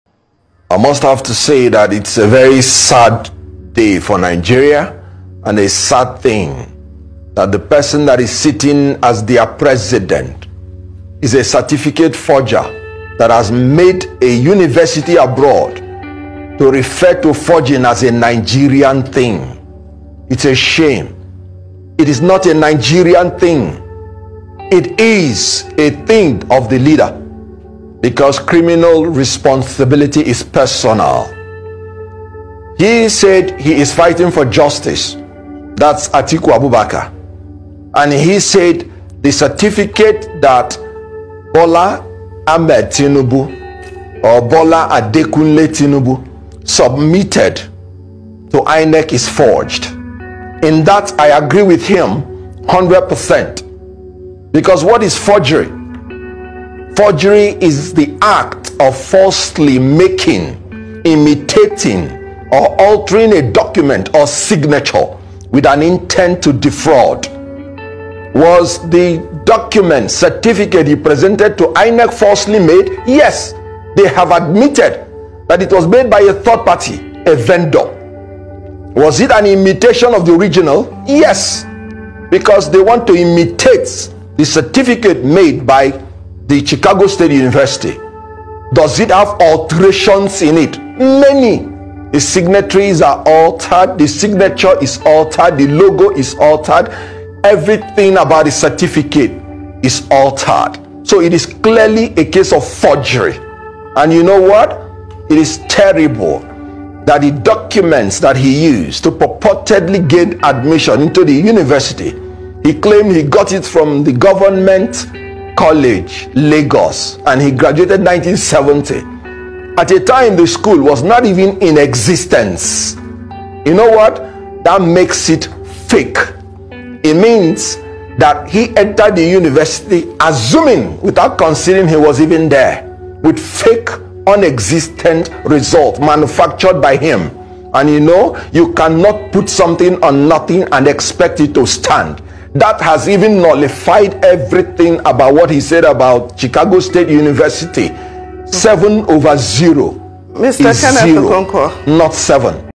😳 😳 Atiku is right, President Tinubu's submitted csu certificate is 100% forged with evidence
this video talks about Tinubu certificate